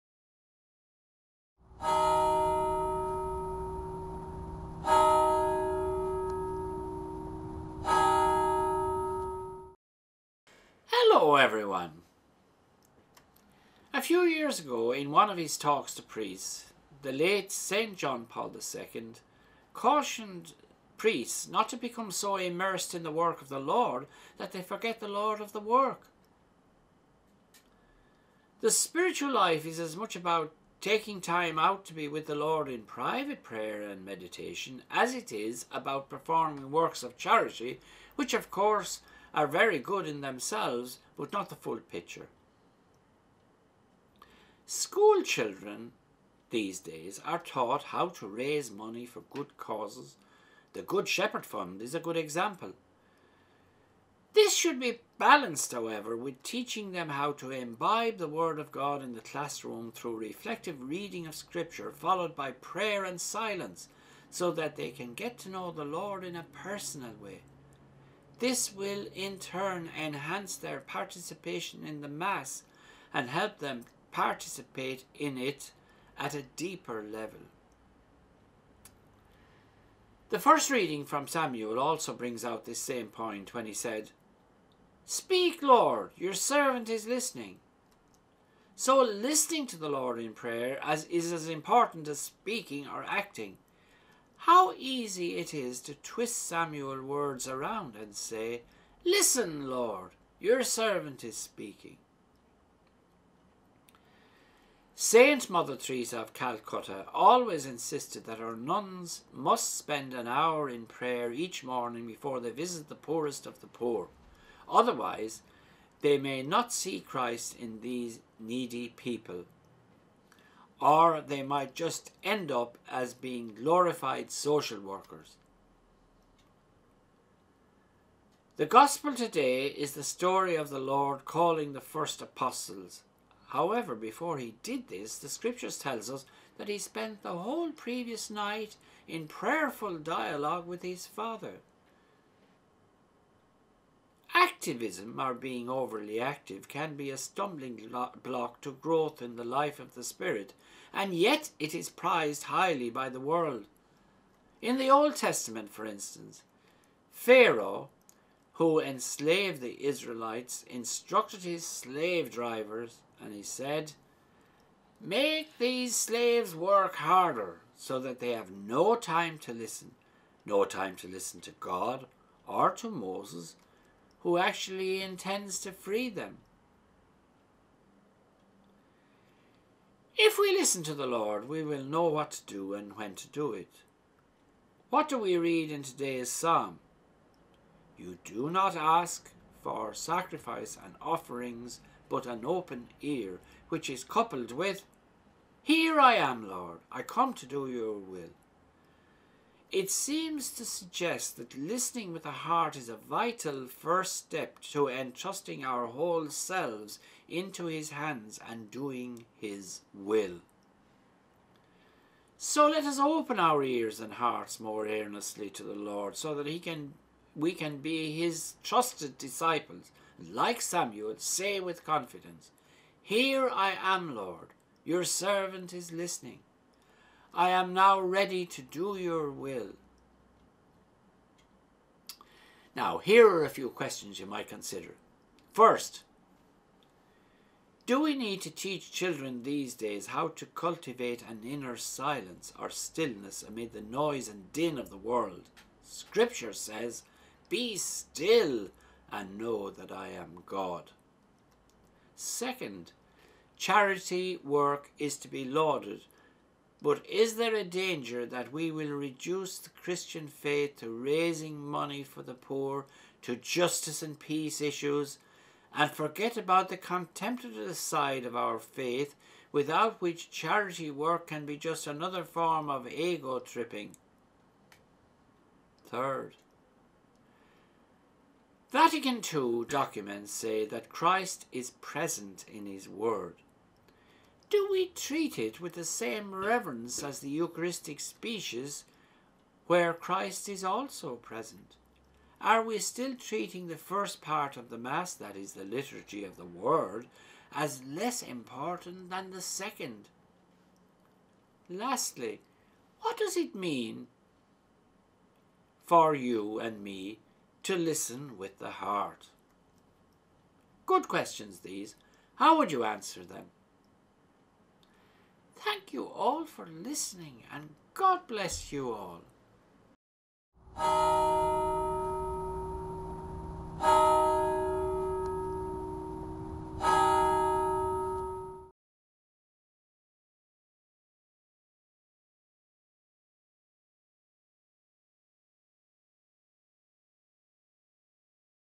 Far From The Madding Crowd
A few years ago in one of his talks to priests, the late Pope John Paul II (now a canonised saint) cautioned them not become so immersed in the work of the Lord that they overlook the Lord of the work.   The spiritual life is as much about taking time out to be with the Lord in private prayer and meditation as it is about performing works of charity which of course is a very good thing in itself, but not the full picture,